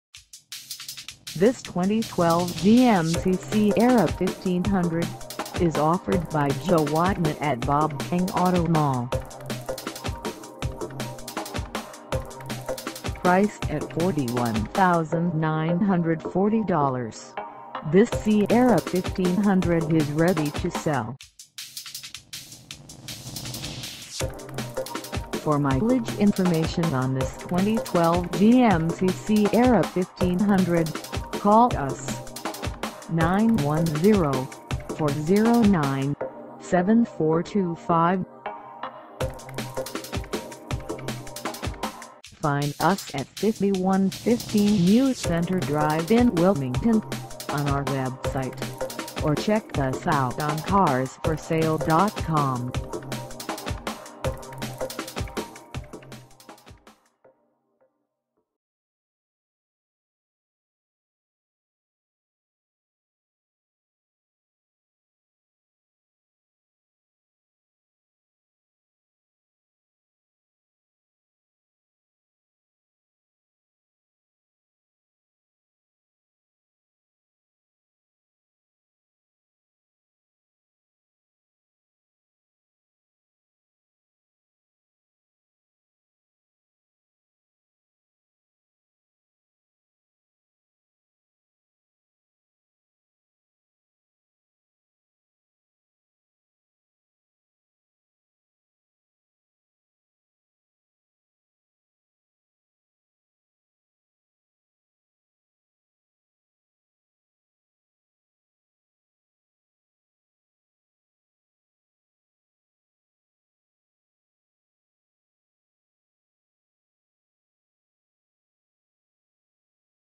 2012 GMC Sierra 1500 SLE - for sale in Wilmington, North Carolina 28403
Joe Wightman at Bob King Auto Mall
5115 NEW CENTRE DR

in Wilmington, North Carolina 28403

Come test dirve this 2012 GMC Sierra 1500 SLE for sale in Wilmington, North Carolina.

Tired of the same dull drive? Well change up things with this outstanding 2012 GMC Sierra 1500. This truck is nicely equipped with features such as Convenience Package (Electric Rear-Window Defogger, Power-Adjustable Pedals For Accelerator & Brake, Remote Vehicle Starter System, and Universal Home Remote), Heavy Duty Cooling Package (Heavy-Duty Air-to-Oil External Engine Oil Cooler), Heavy-Duty Trailering Package (Heavy-Duty Rear Automatic Locking Differential), Off-Road Suspension Package, Power Tech Package (Bluetooth For Phone, Dual-Zone Automatic Climate Control, and Steering Wheel Mounted Audio Controls), Skid Plate Package, 6-Speed Automatic, 4WD, Ebony w/Leather-Appointed Front Seat Trim, Inside Rear-View Mirror w/Rear Camera Display, Integrated Trailer Brake Controller, and Rear Vision Camera. Climb into this superb Sierra 1500, knowing that it will always get you where you need to go, on time, every time.